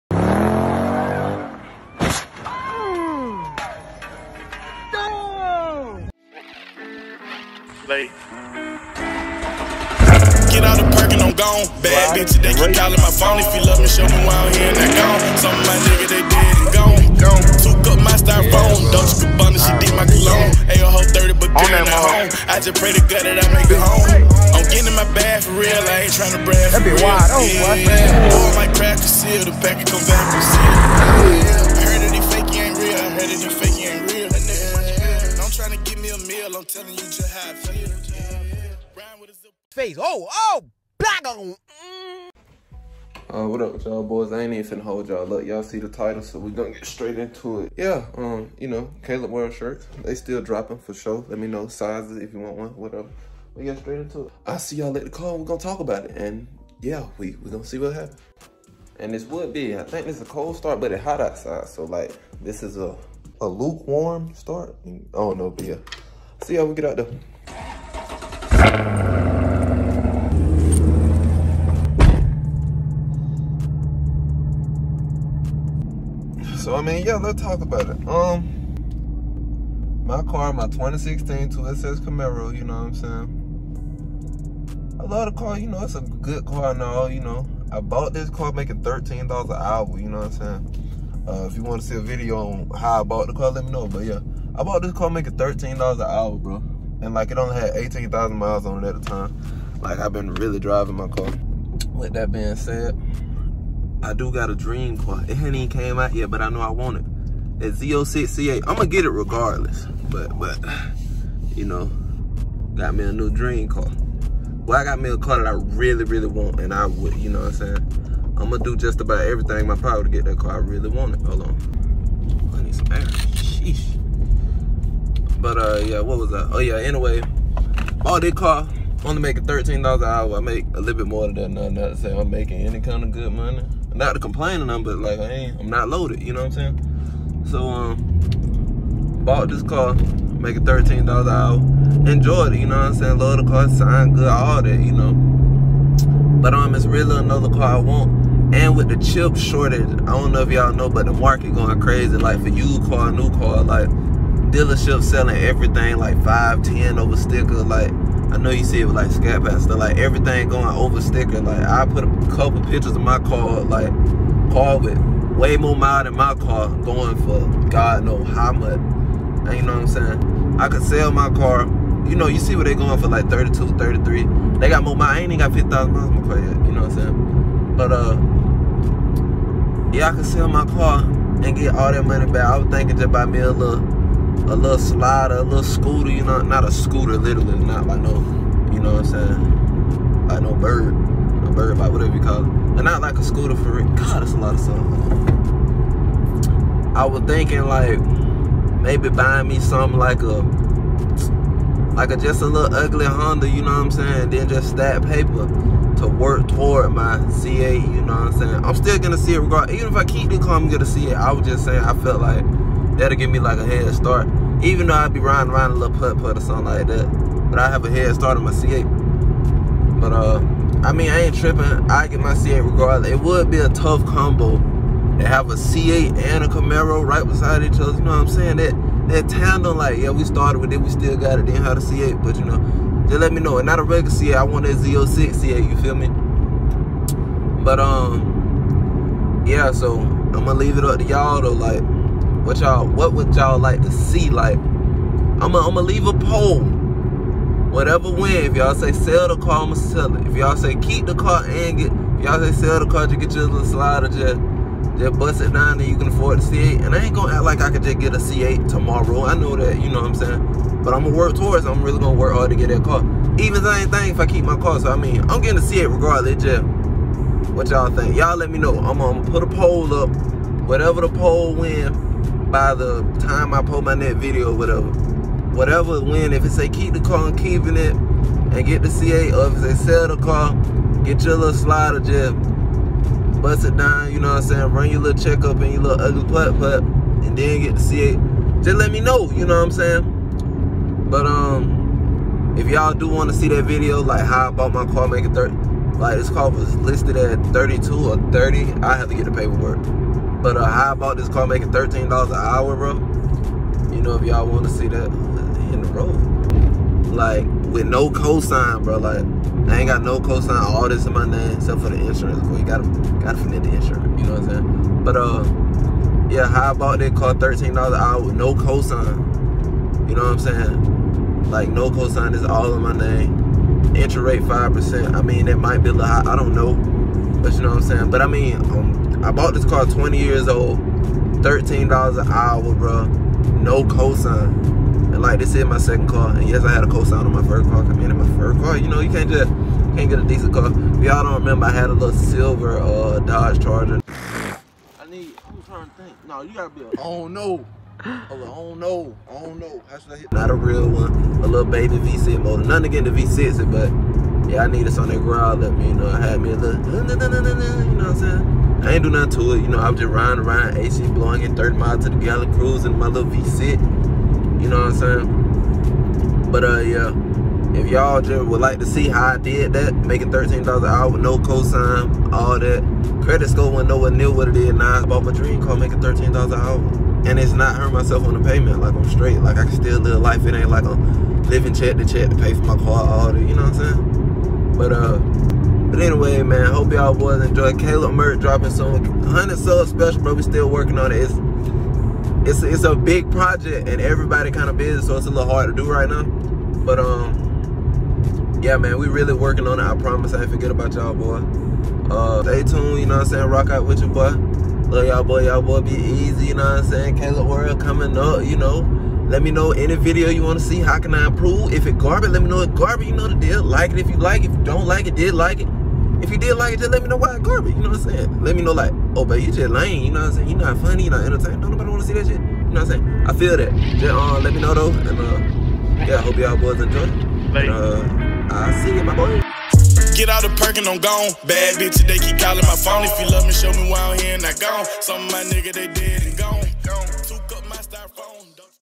SHOULD I SELL MY DREAM CAR?!?!(2016 CAMARO 2SS)
I need everyone's help on making this decision. I still enjoy and have fun with this car, but the car market right now is very profitable. Either way this video will be used to determine how I go about my car. Whether its keeping it or selling it. However it turns out I'll be fine with both. Although if the poll does fall in favor of me keeping it. January I will have it sent to a shop who personalizes in Camaros, and have them upgrade the performance on my car.  The Corvette C8 still will be purchased regardless of anything I do from this point on. Thank You All for your continuous support!!

@calebwrld on all Socials